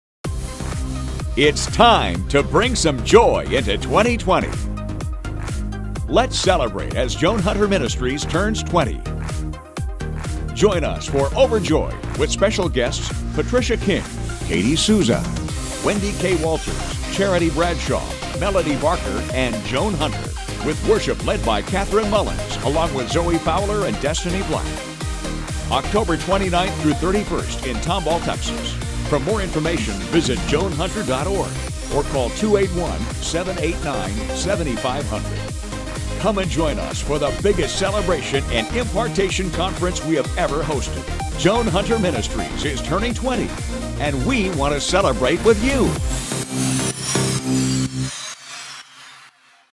Overjoyed! Anniversary Celebration Announcement
Joan Hunter Ministries is turning 20, October 30th, 2020! We are OVERJOYED to celebrate God's goodness, favor and miracles! Come and join me for the biggest celebration and impartation conference we have ever hosted!

Speakers include Patricia King, Katie Souza, Wendy K. Walters, Melody Barker, Charity Bradshaw and Joan Hunter. Seven sessions + video appearances by Doug Addison and Dr. Clarice Fluitt.